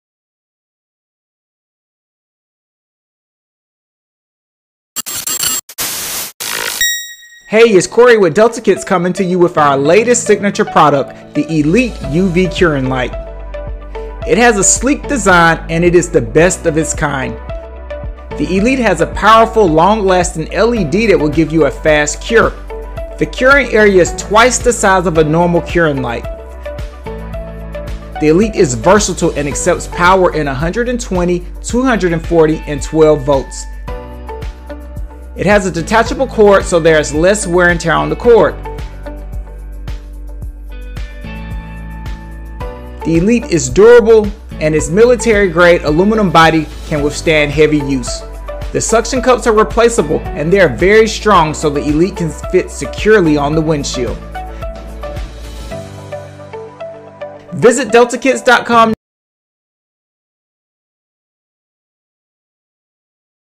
Elite UV Curing Light | Delta kits | Jaags
Constructed of a sleek, new aluminum body design with an anodized coating, Delta Kit’s all-new Elite 365 nm Curing Light is more durable, longer-lasting, and 3 times faster than competitive models!
More cool features of the Elite Light:
Durable, lightweight, and long-lasting
Cooling fins to dissipate heat
Ultra-secure replaceable suction cups
Fuse-protected 12V plug
10’ cord
12V, 120V, 240V capable*
50% larger curing area than previous models
On/Off switch